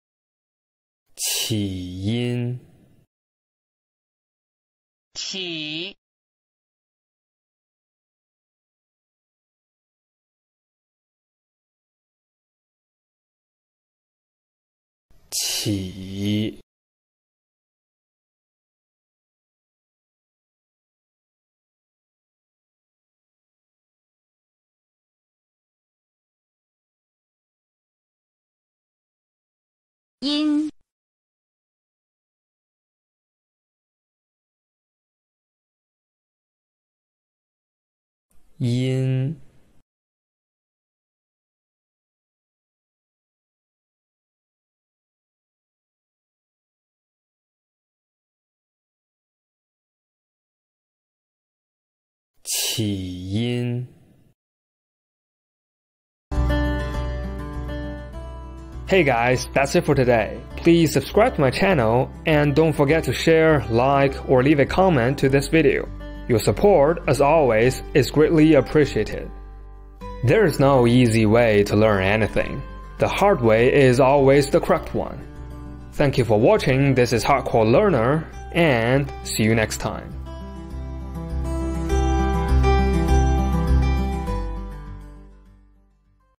Learn to speak and write Chinese 76, 起因, Cause
Learn how to speak and write Chinese in just 1min every day, with clear stroke sequence and various calligraphy for each of the character.